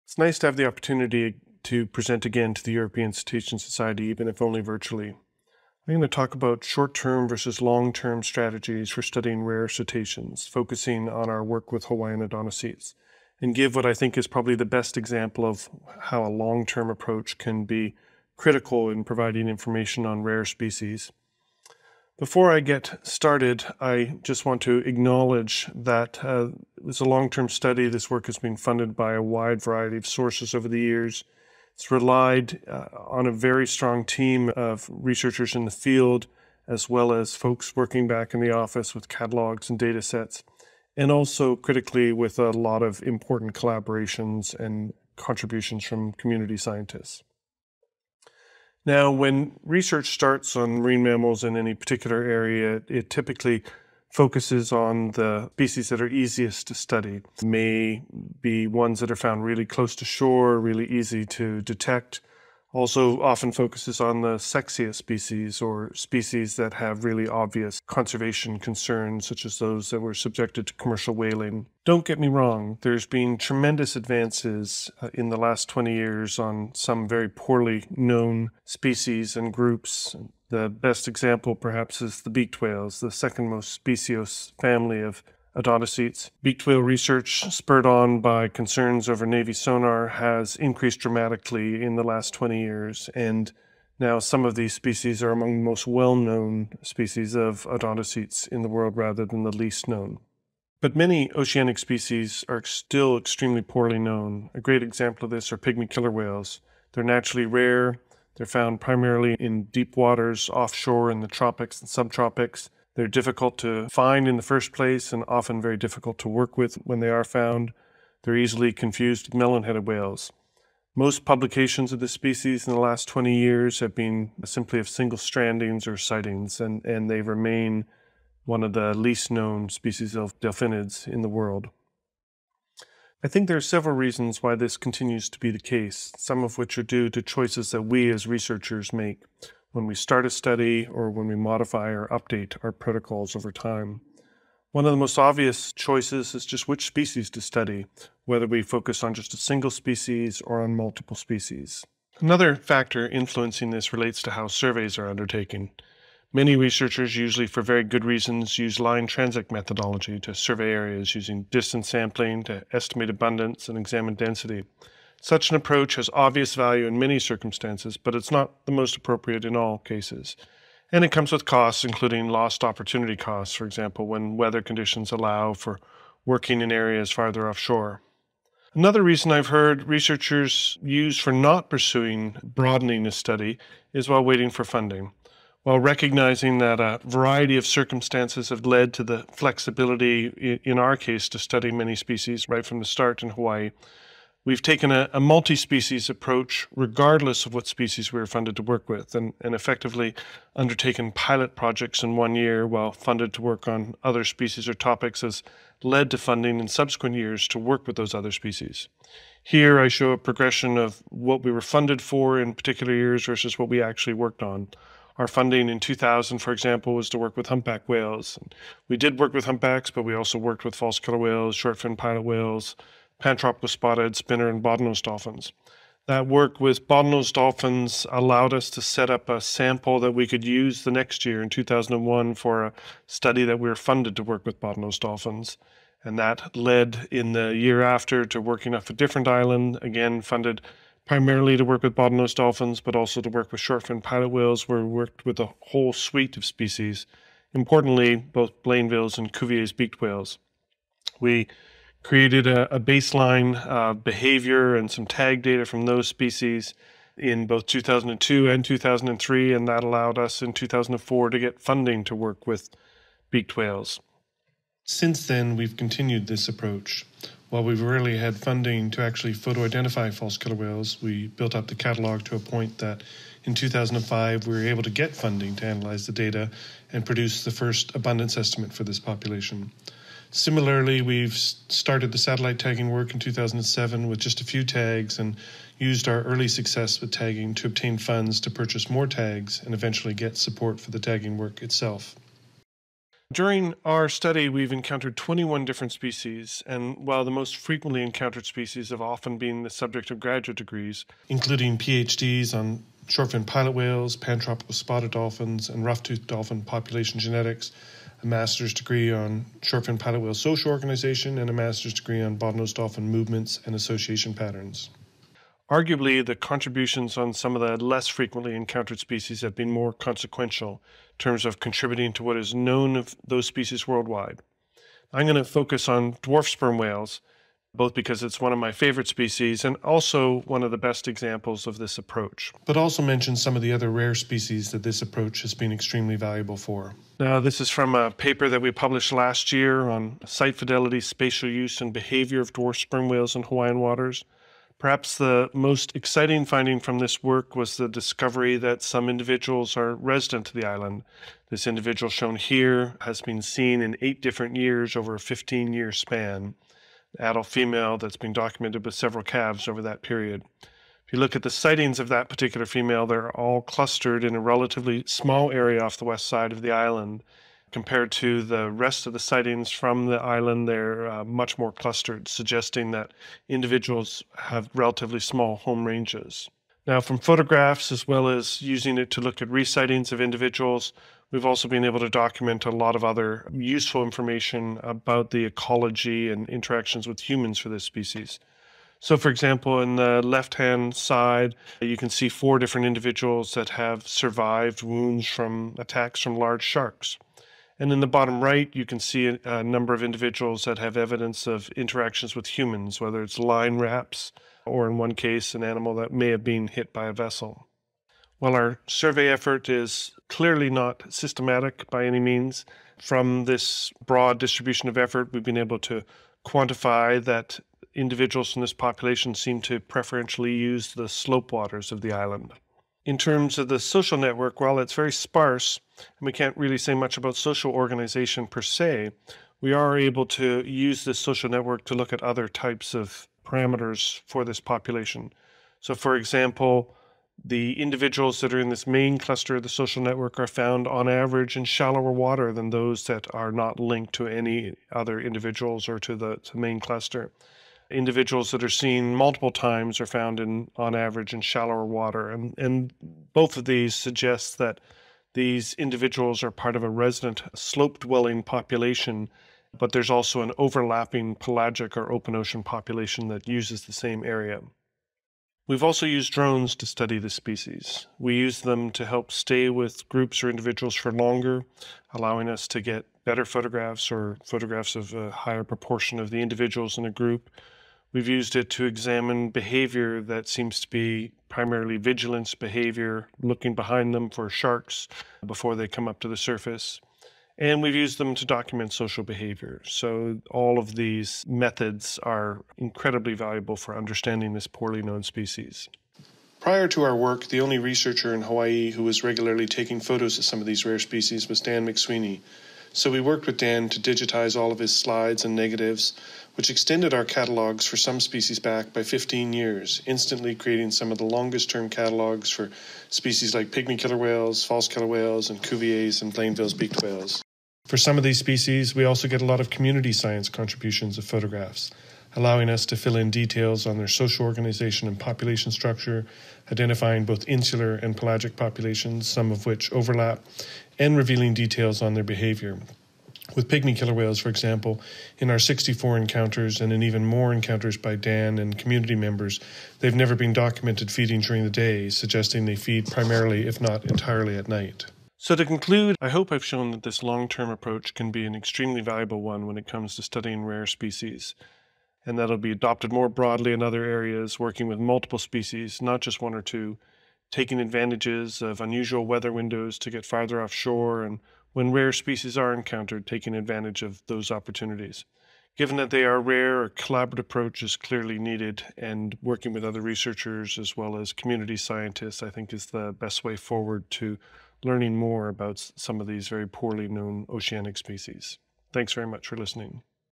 Robin Baird - ECS 2022 - Short-term versus long-term strategies for studying rare cetaceans
Robin Baird presenting at the 2022 European Cetacean Society Conference

Abstract:
Cetacean research efforts have traditionally focused in nearshore areas on species that are easy to study - those that are relatively common and/or are relatively approachable. Species that are difficult to find or approach are often ignored or left quickly even when they are encountered, and survey designs often constrain flexibility to venture offshore when conditions allow. While such short-term approaches may make sense for graduate students or meet the expectations of funding agencies, they are not effective for increasing knowledge and identifying conservation concerns of rare and/or difficult-to-study species. Since 1999, a study of odontocetes around the main Hawaiian Islands has taken a different approach. First, survey efforts have been undertaken as far offshore as possible when weather conditions are favorable. Second, regardless of the target of funding support, sampling priorities are in inverse proportion to encounter rates - rare species, when encountered, are always the priority. Given they are rare, there is little lost opportunity cost in terms of working with more common species that are often the focus of funding agencies. Combined with citizen science contributions and opportumistic photos taken by other researchers, in
the long-term this approach has been extremely productive, leading, for example, to the identification of small resident populations of pygmy killer whales, dwarf sperm whales, and false killer whales. Hawaiian populations of Blainville's and Cuvier's beaked whales, rough-toothed dolphins, and melon-headed and short-finned pilot whales are now among the best-known populations of these species worldwide. Unfunded "pilot" studies of rare species have often led to funding, but such funding, or lack thereof, should not be a factor in species sampling priorities. Given the rarity of sightings of many of these species, and thus slow accumulation of information, collaboration with other researchers and citizen scientists is critical for the success of this approach.